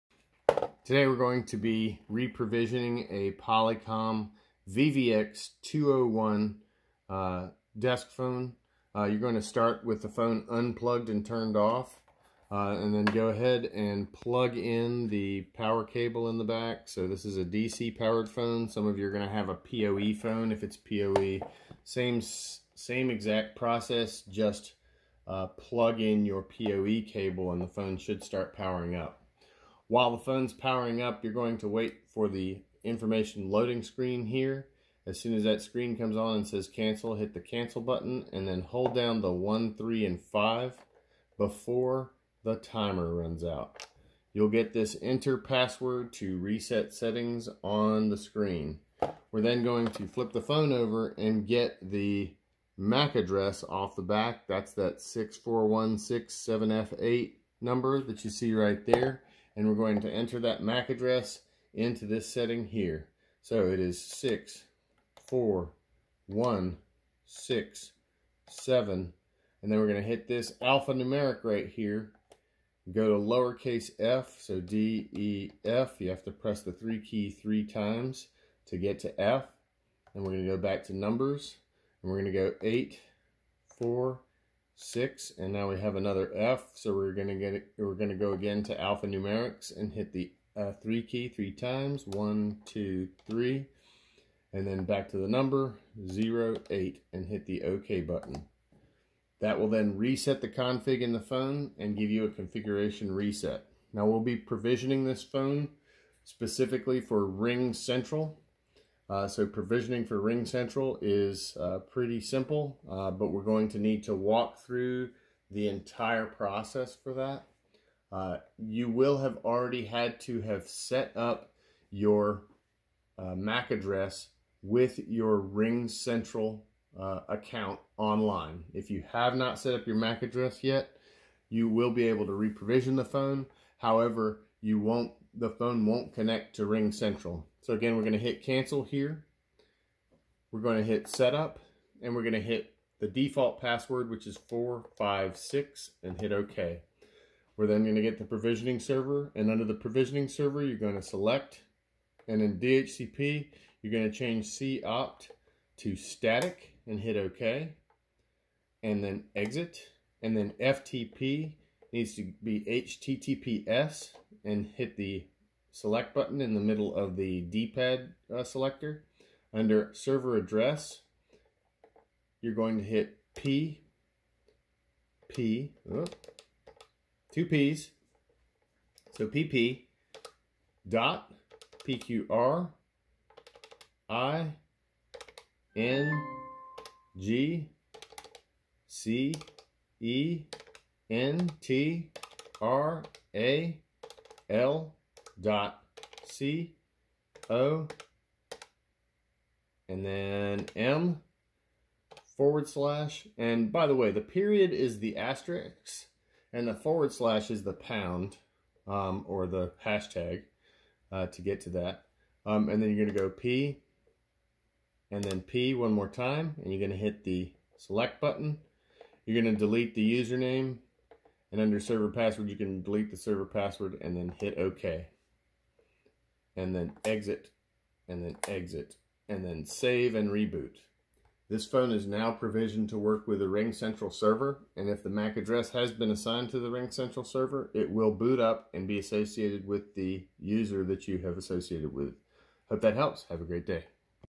How to factory reset polycom phones if you don't have the password
Use the method in the video and your can reprogram any polycom phone no matter which provider currently has the phone locked. This works on all vvx and sound point models.